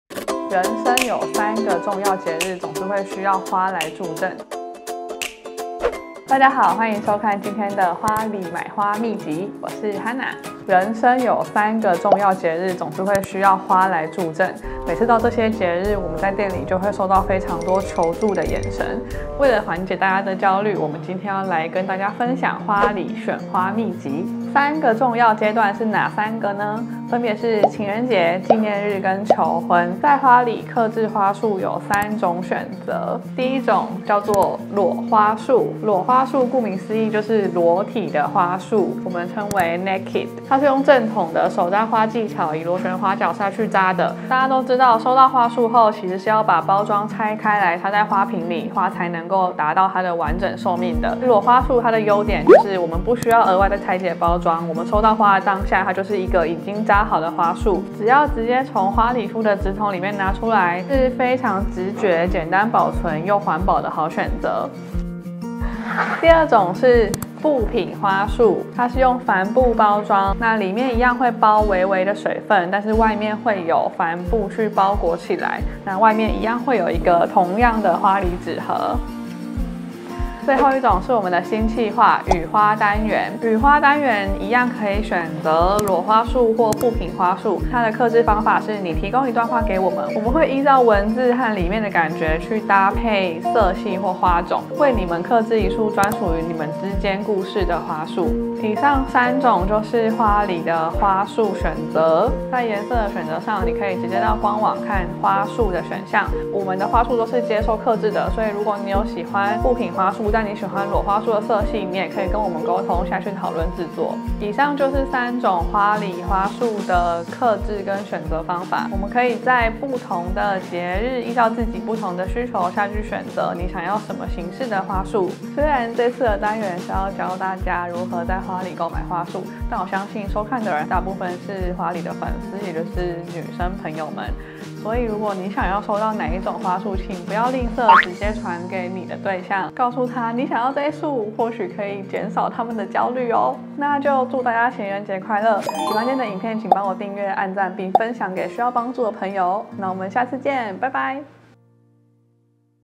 情人節選花說明書，讓我住進你心裡💕｜花藝教學｜花裡 FORi Flower
是不是剛好，你正在找一束花？

曖昧階段的你 _ 需要一點確定☺️
在熱戀期的你 _ 需要一點浪漫🥰
老夫老妻的你 _ 需要一點驚喜🥳

每個人都喜歡被重視的感覺，
相信我，一束花可以讓你的生活很不一樣！

趁這個特別的節日，
讓兩顆心之間的距離更靠近！